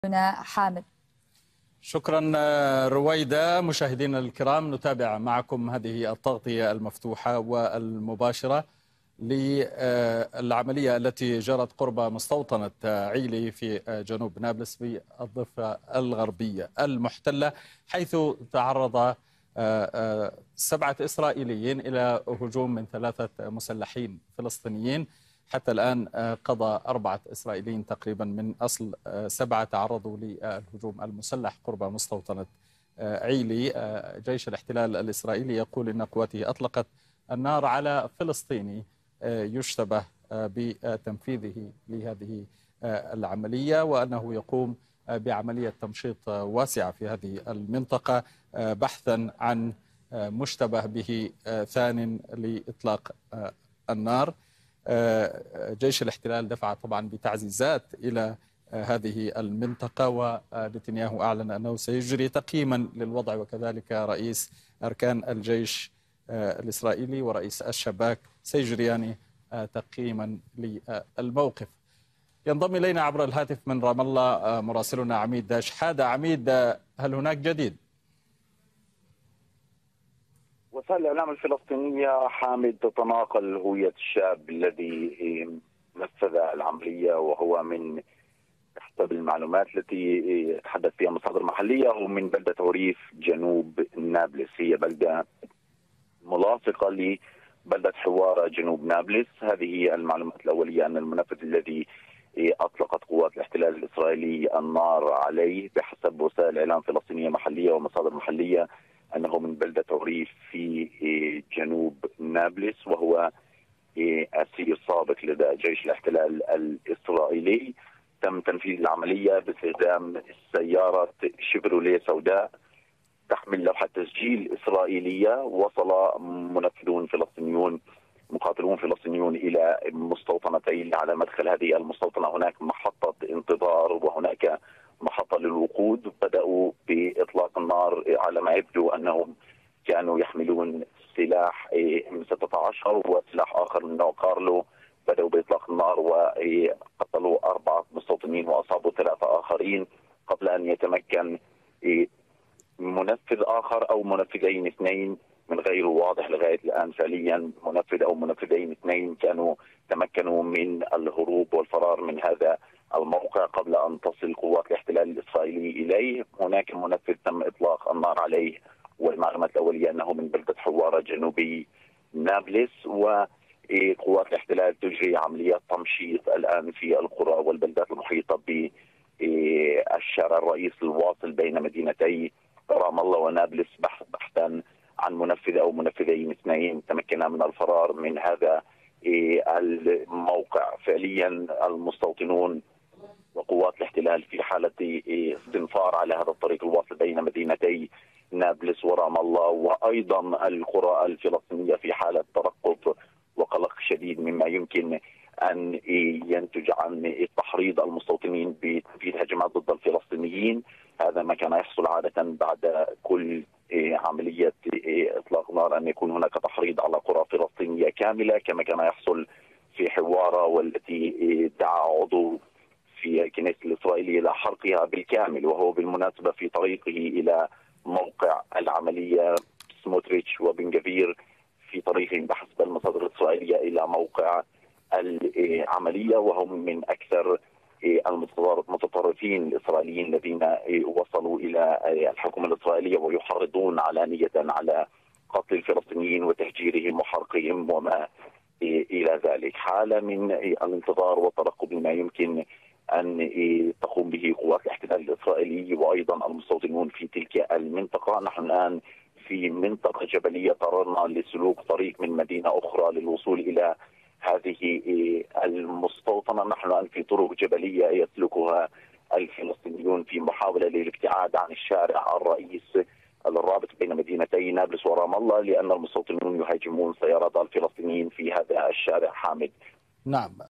نابلس.. هوية الشاب منفذ عملية مستوطنة "عيلي".. تفاصيل أوفى مع مراسل التلفزيون العربي
قال مراسل التلفزيون العربي عميد شحادة إن المعلومات الأولية تشير إلى أن منفذ عملية مستوطنة عيلي هو من بلدة حوارة جنوبي نابلس مع مواصلة قوات الاحتلال البحث عن مساعديه حيث تمت العملية عبر سيارة بلوحة منجمية إسرائيلية أسفرت عن مقتل أربعة مستوطنين وإصابة أربعة آخرين.